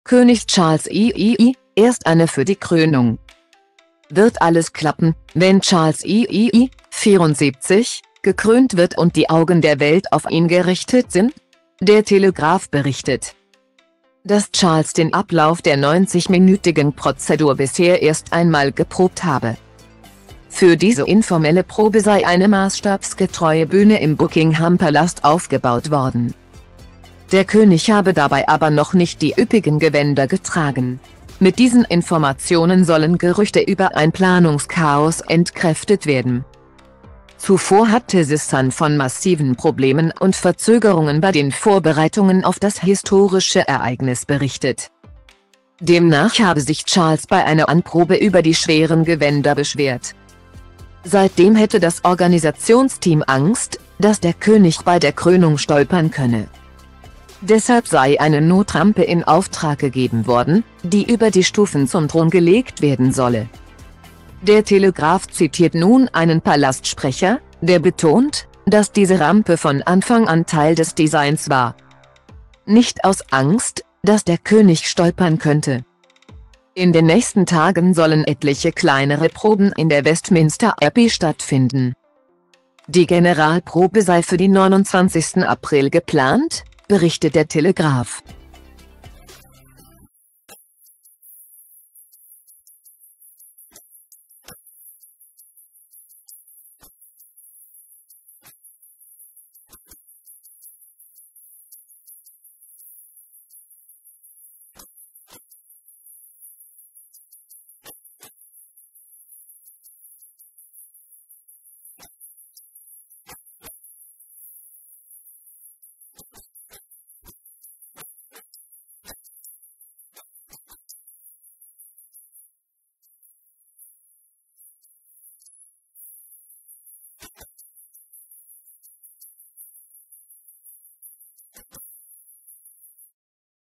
König Charles III.: Erst eine für die Krönung
Wird alles klappen, wenn Charles III. (74) gekrönt wird und die Augen der Welt auf ihn gerichtet sind? Der "Telegraph" berichtet,dass Charles den Ablauf der 90-minütigen Prozedur bisher erst einmal geprobt habe.Für diese "informelle Probe" sei eine maßstabsgetreue Bühne im Buckingham-Palast aufgebaut worden.